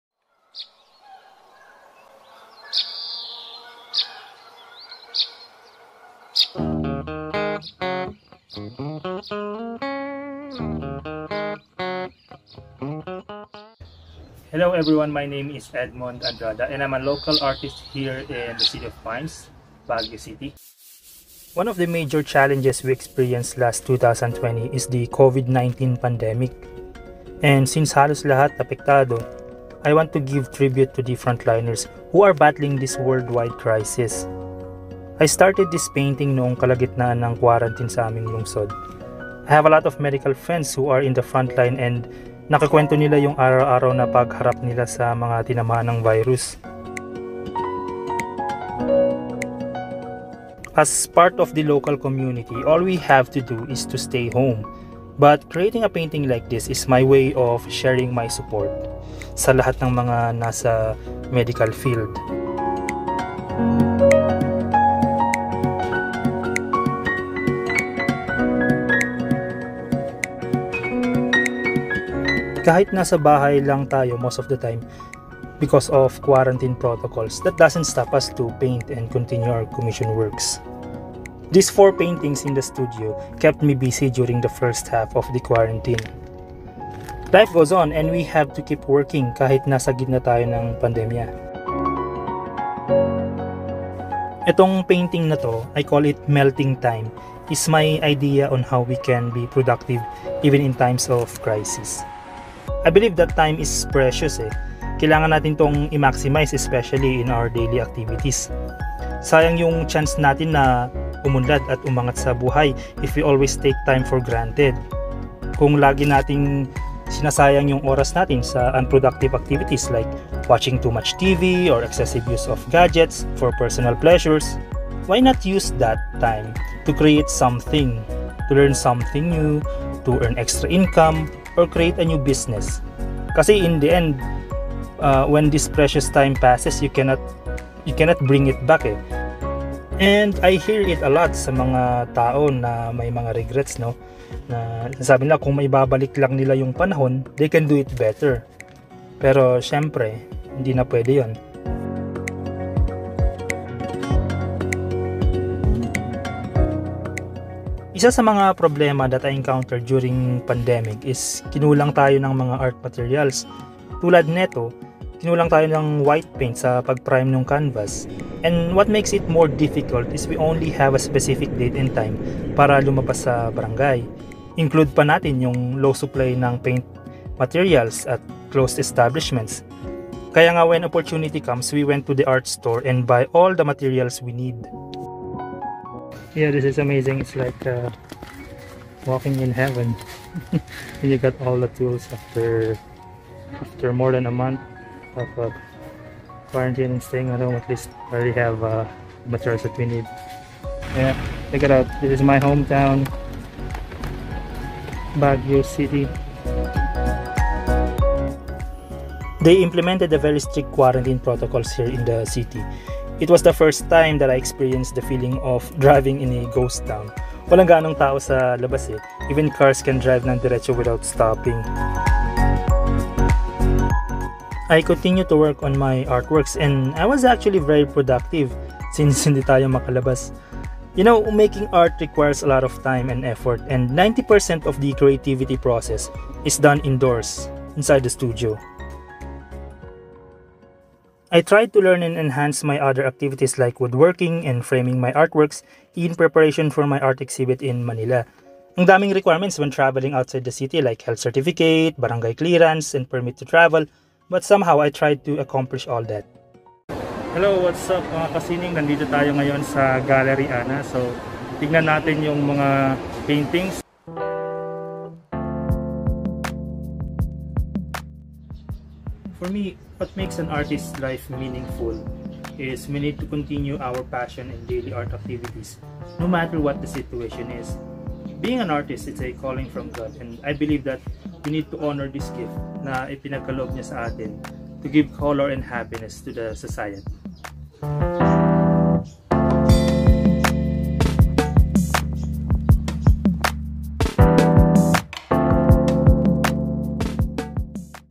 ART MUST GO ON | Painting Documentary in the Midst of Covid-19 Pandemic
My entry.
Sineng Sining for Bagong Biswal, National Arts Month 2021

Here's a video documentary of my previous artworks and activities that was made during the COVID-19 crisis 2020. Included in the video are situations of road conditions in Baguio.

Check his music! His works are really great and amazing!